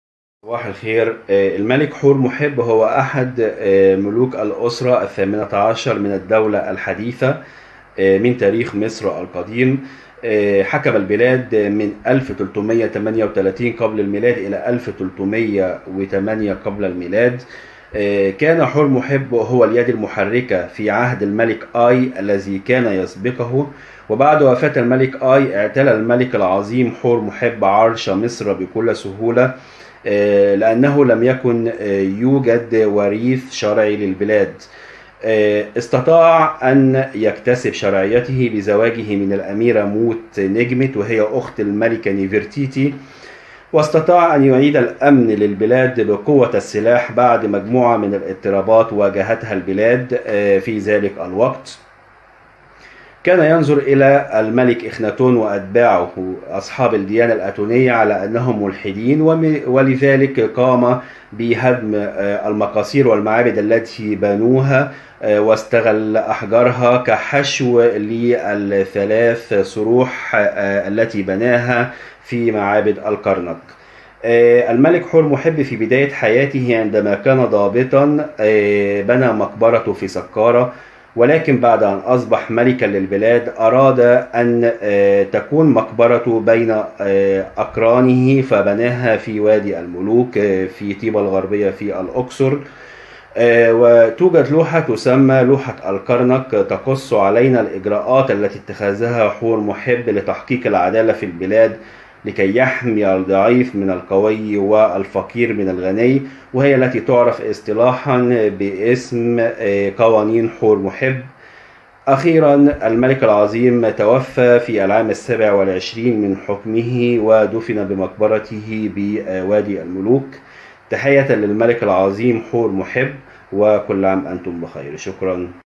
الملك حور محب
معلومات مختصرة فى جمل بسيطة عن الملك حور محب